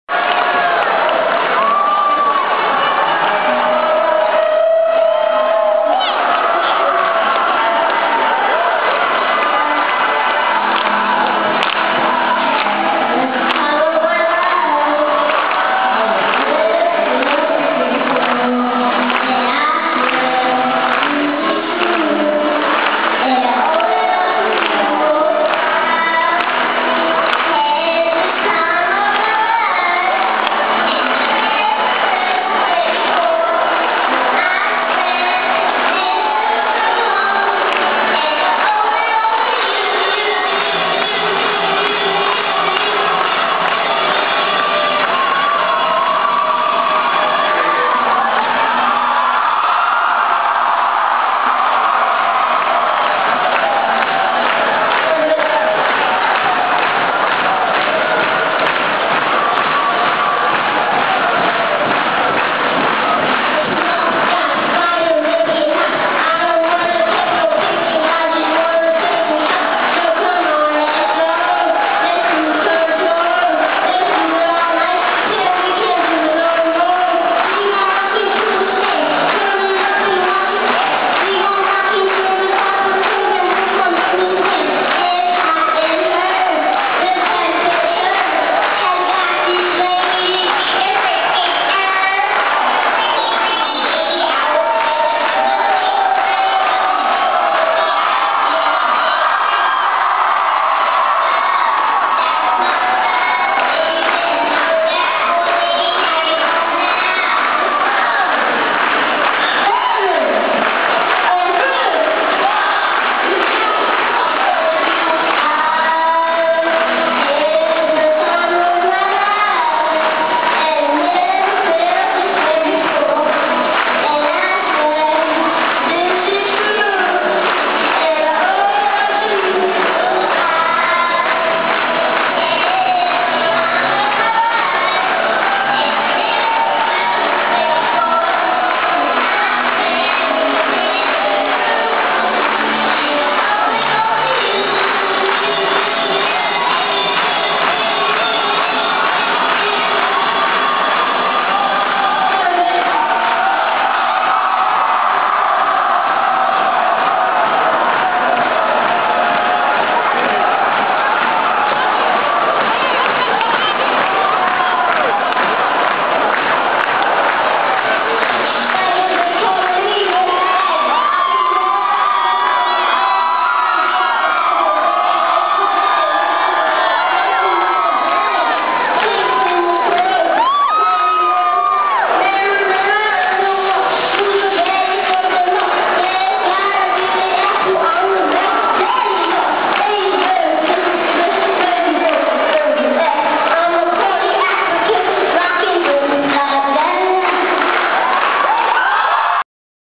Tucker Hass singing at FTK PEP rally
For the kids or (FTK) for short is run by the 4 diamonds fund to raise money for kids who have a sever illnesses for the PENN STATE HERSHEY MEDICAL CENTER.